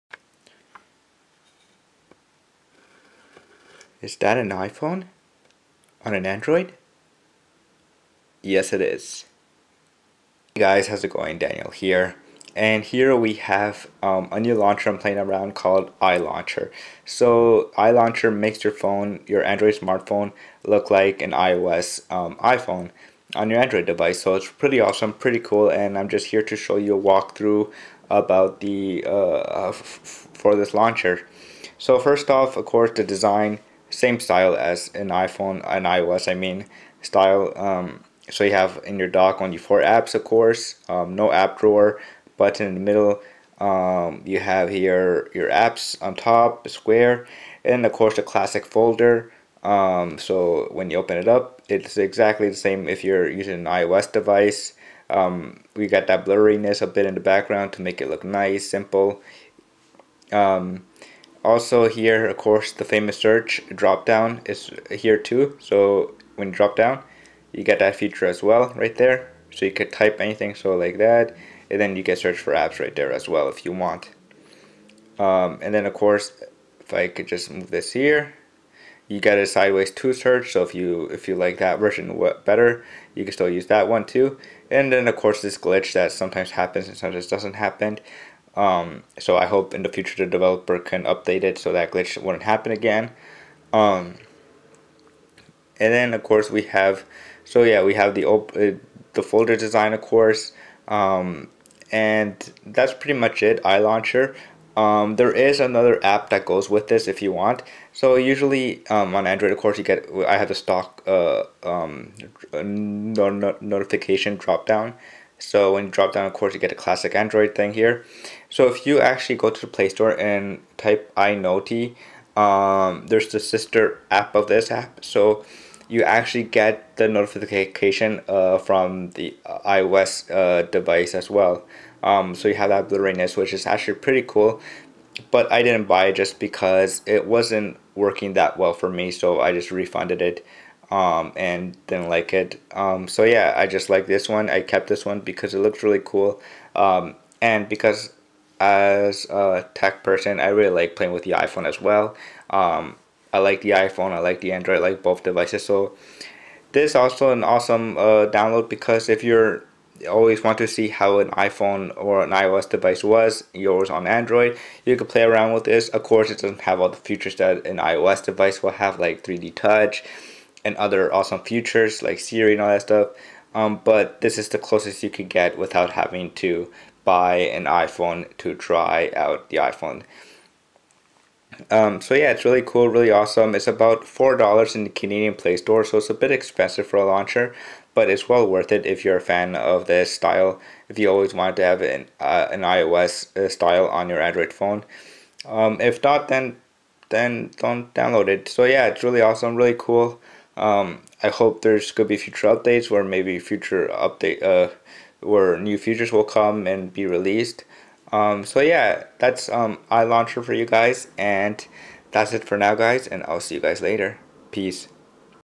iOS On My Android Smartphone
iLauncher will transform your android smartphone with iOS style.
iNoty is the sister app to this app.